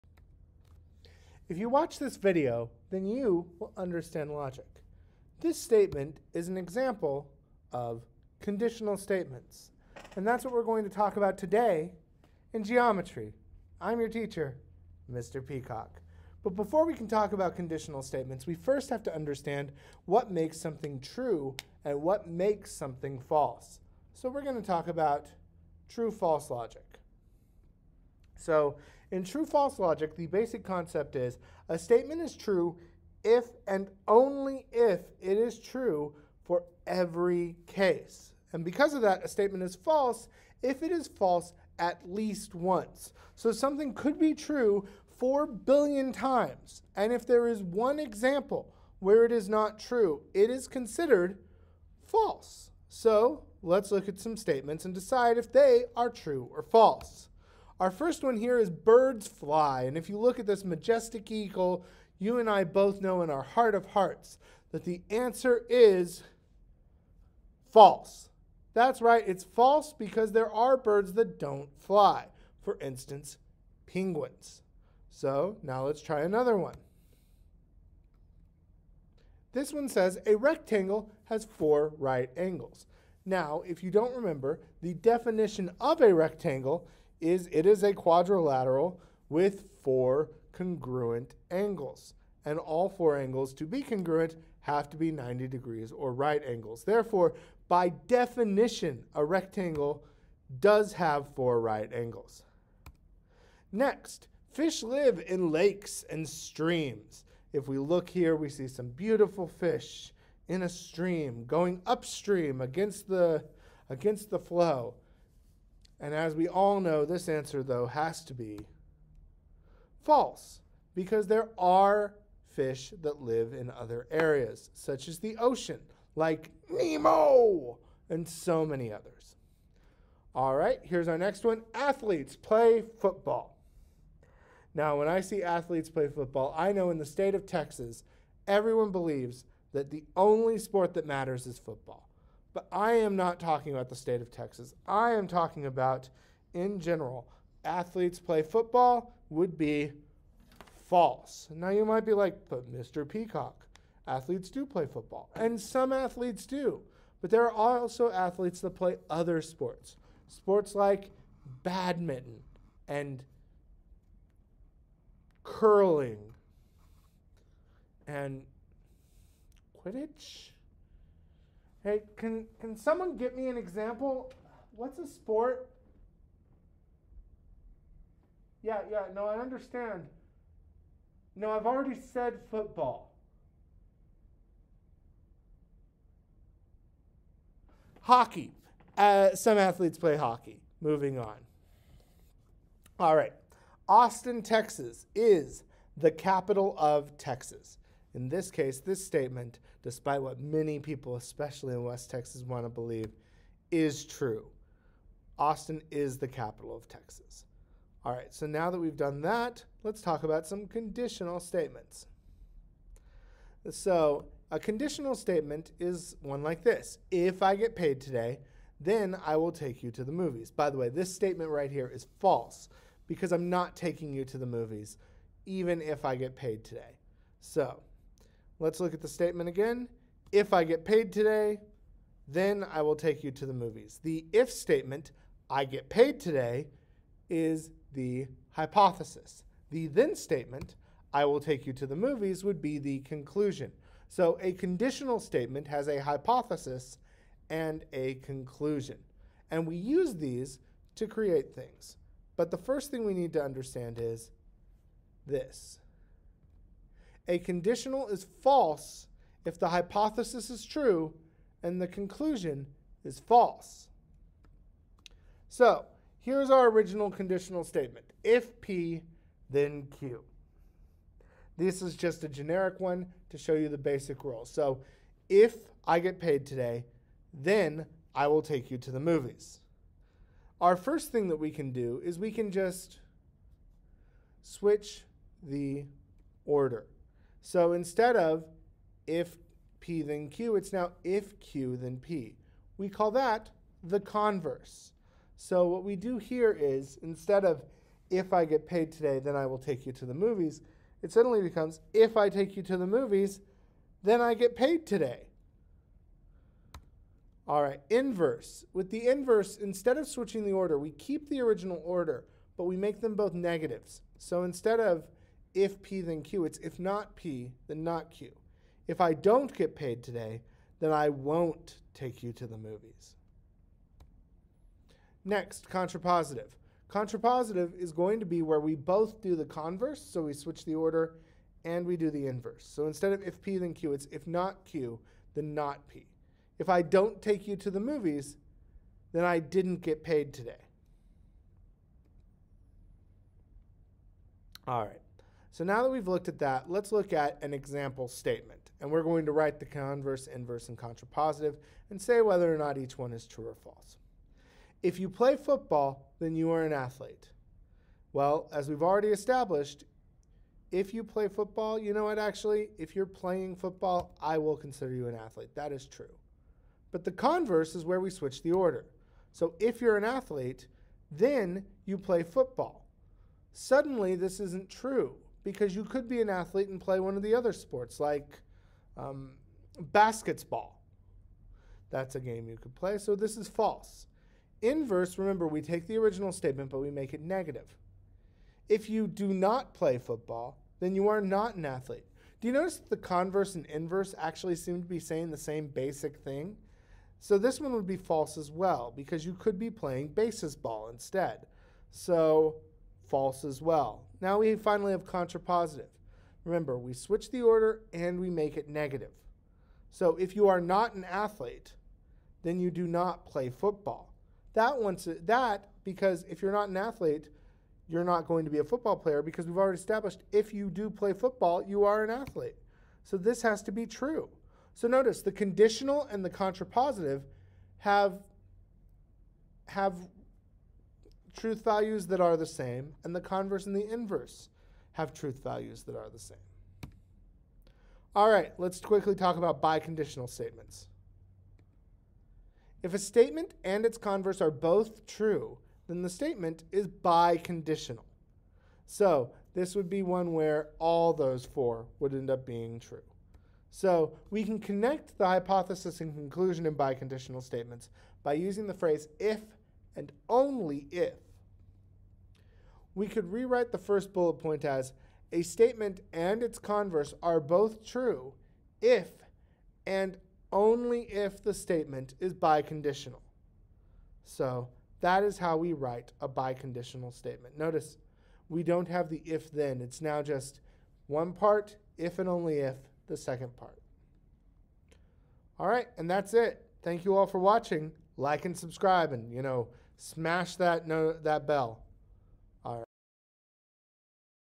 Geometry - True/False Logic and Conditional Statements
In this lesson, we learn about how to know if something is true or false as well as conditional and biconditional statements!